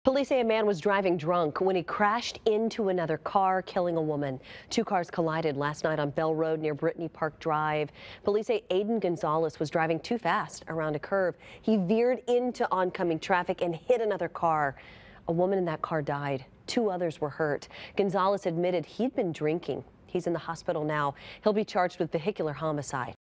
Man faces charges after deadly crash
Police said the suspect was found lying on the ground at the scene and he admitted to drinking five beers.